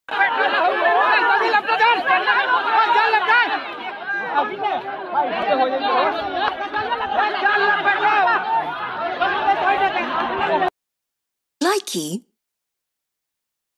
Pangolin/wild bon rou
A pangolin in a angry state